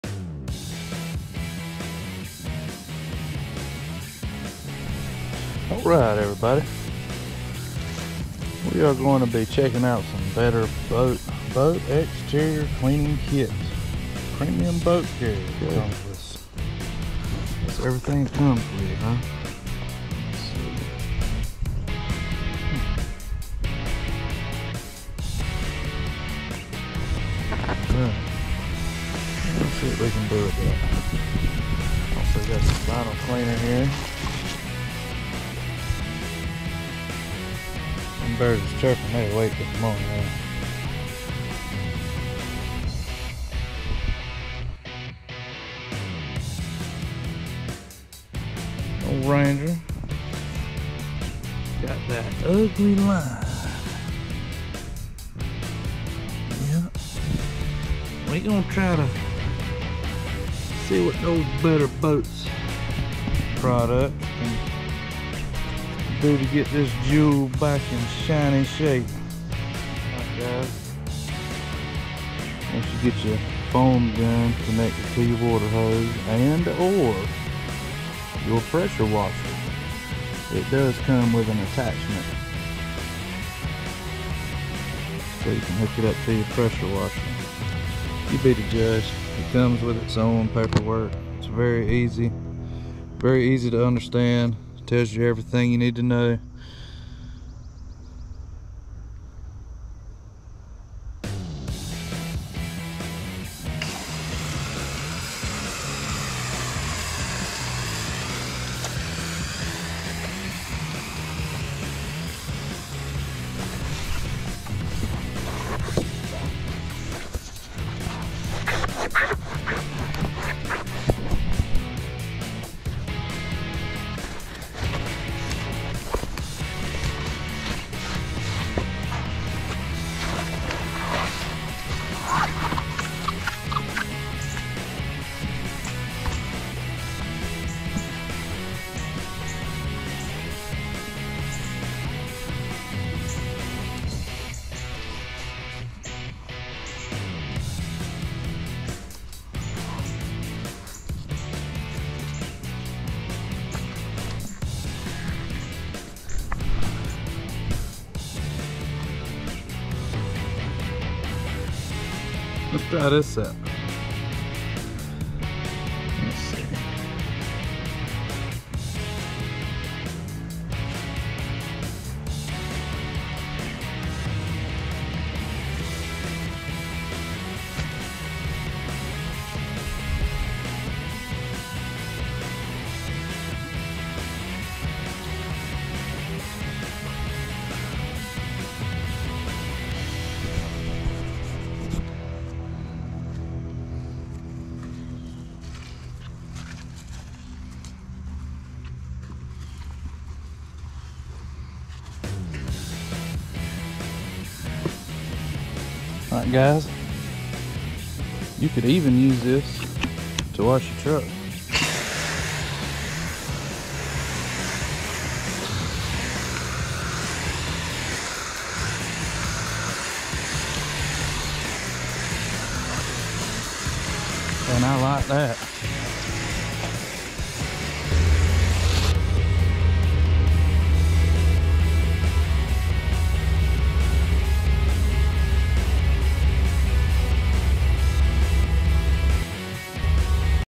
Better Boat Exterior Boat Cleaning Kit With Vinyl Cleaner Review
Whats up Everyone. I hope yall are staying cool out there. I was recently asked to do a video review on Better Boat products, and i chose to review the Better Boat Boat Exterior Cleaning Kit and i wanted to try to Vinyl Cleaner for the drink stain in my seat and my cup holders. I was impressed with the Bio Degradable Soap, was not expecting it to remove the water line as good as it did. I haven't washed my boat in 10 trips i normally try to keep it clean ever other trip but with summer time here i got busy with the kids being out of school and life i lost track. So Today i washed her up and done a little video showing off the products with the final result being just what it needed to be !!

Boat Exterior Cleaning Kit (6 piece kit)
 Foam Gun with adapter fitting for some pressure washers
 1 Bottle Of Concentrated Bio Degradable Soap
 1 Large Sponge
 2 Micro Fiber Cleaning Cloths
 1 Scuff Eraser